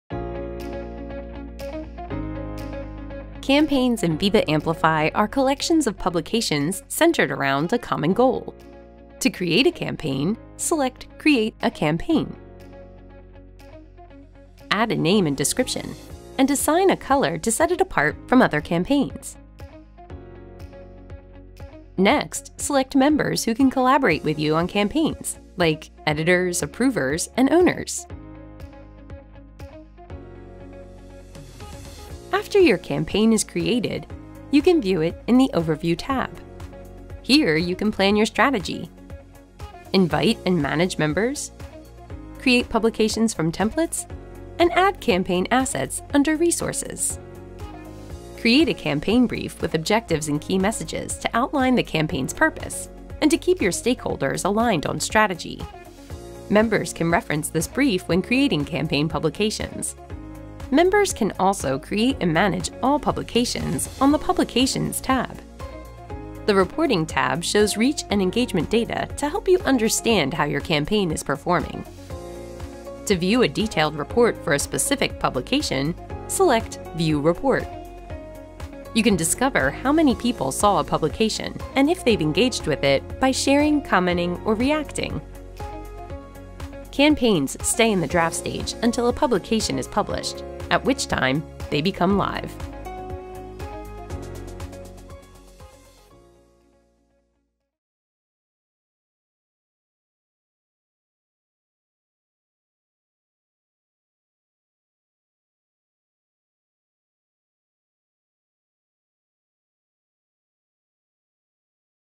Create a campaign in Microsoft Viva Amplify
📺 Create a Viva Amplify campaign that provides one central place for you and your campaign members to plan and build engaging content, work on campaign initiatives, and collaborate from anywhere.